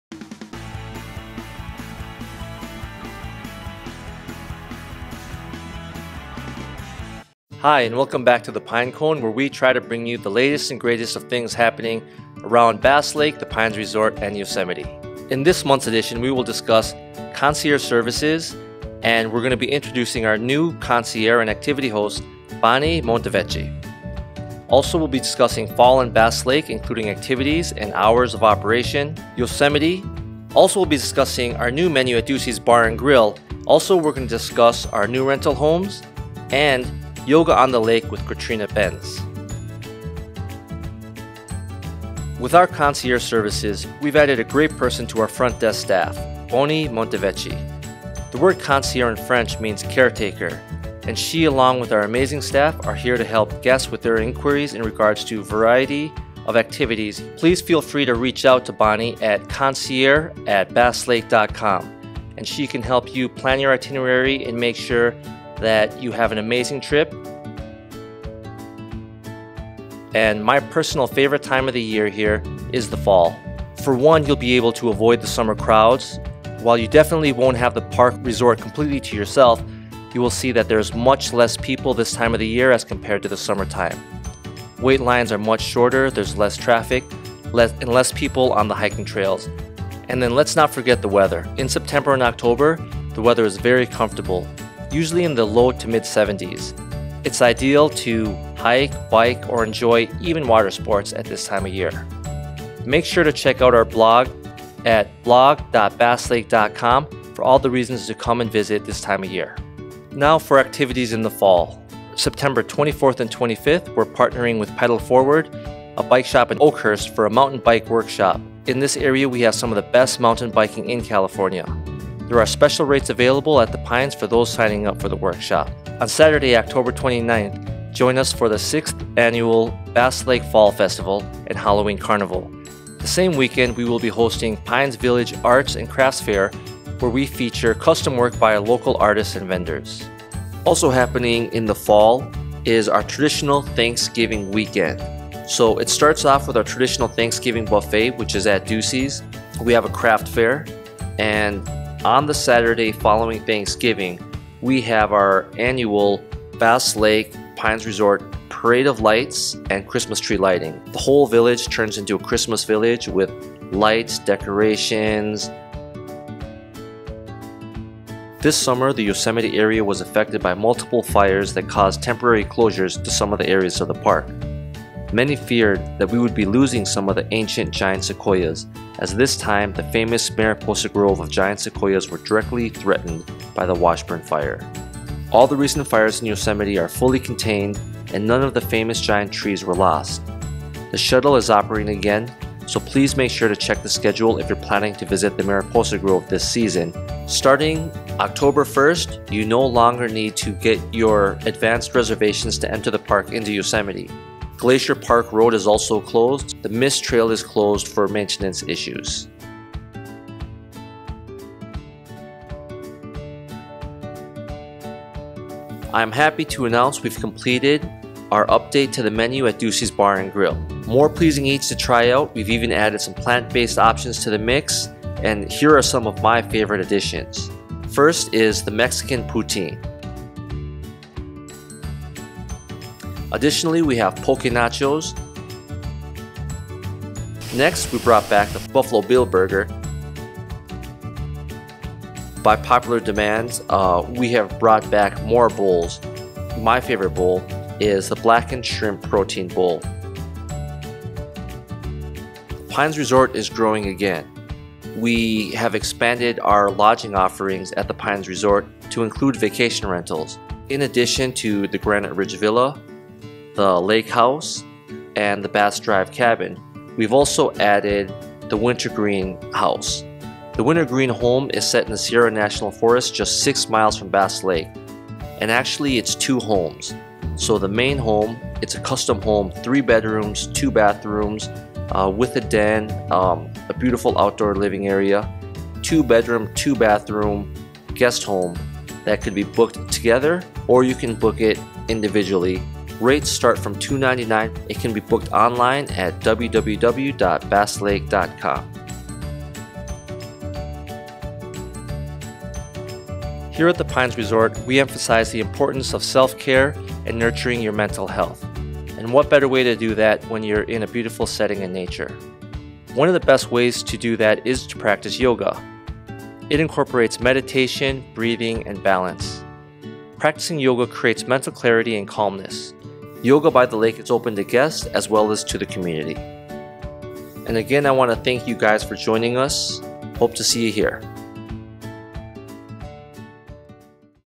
The Pine Cone - Newsletter (September 2022)
Welcome to the Pine Cone Newsletter, where we bring you the latest updates from Bass Lake, Yosemite and surrounding areas.

In this month’s edition we will discuss:
1. New service at the Pines Resort
2. Fall in Bass Lake including activities and our hours of operation
3. Update from Yosemite
4. New menu at Ducey’s Bar & Grill
5. The Pines is Growing again!
6. Yoga at the Lake

We hope so you at the Lake!